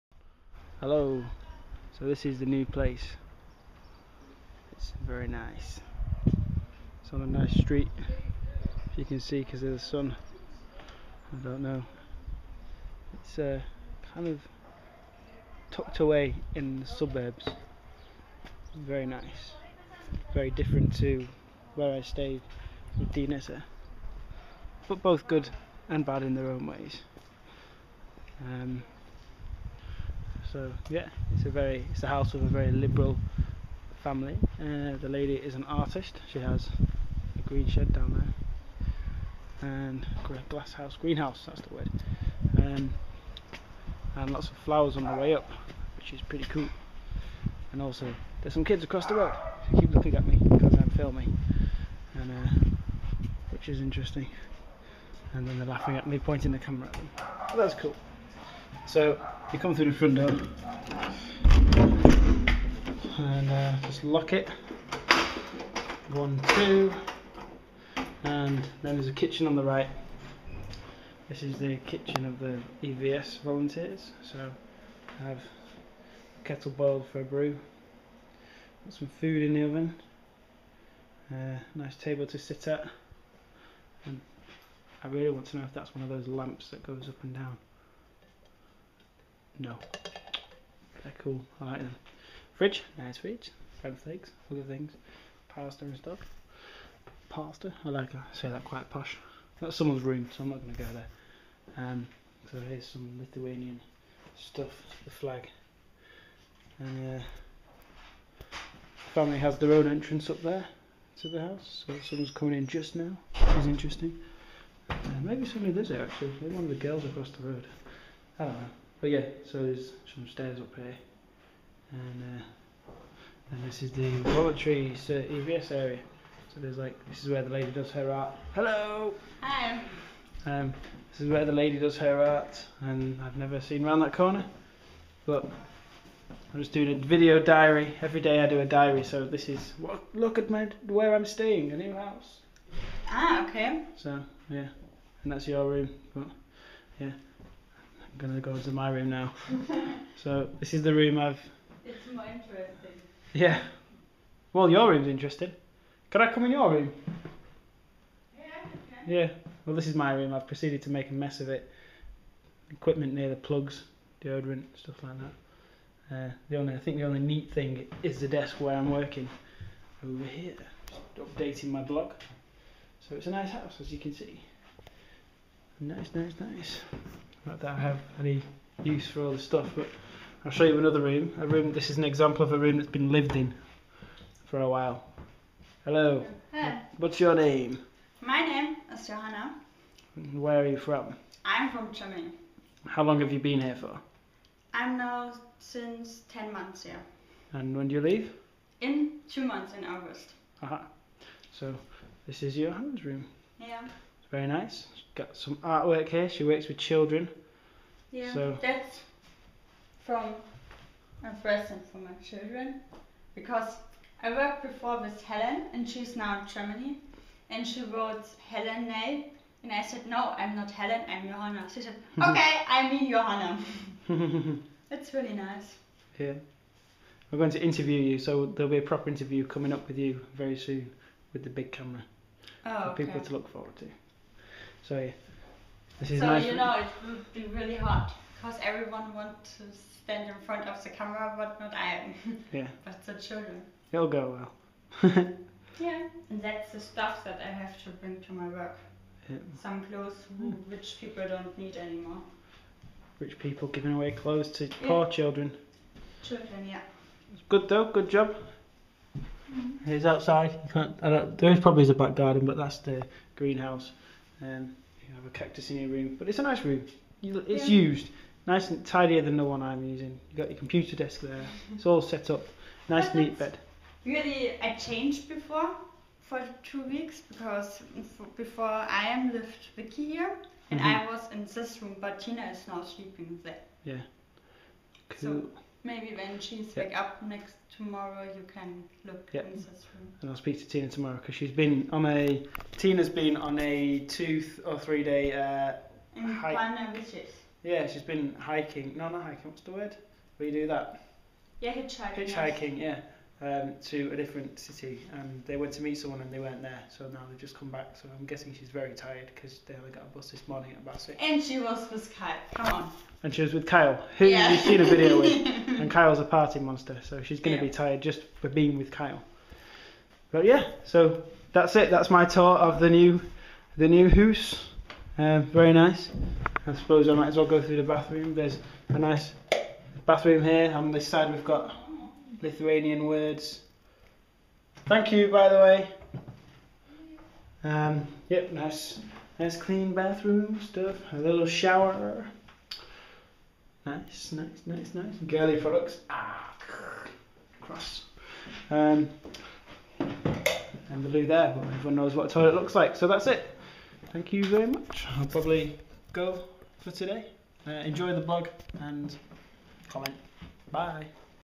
16 Days in Lithuania, Day 10 - Mike Leigh Cooper, UK Reporter for EU Relay Project
Welcome to a parallel universe, kind of. Or at least it feels a bit like that!

On day 10 I moved out of the flat with Maurizio and into a house with some EVS volunteers who work with Lithuania's largest humanitarian organisation, Caritas.

In this video I give a little tour around my new place - what do you think?